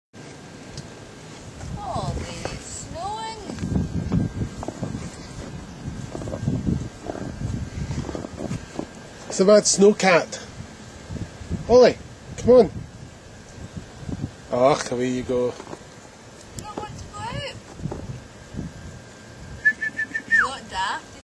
Snow Cat
Well ok, it's a ginger moggy who's never seen snow before!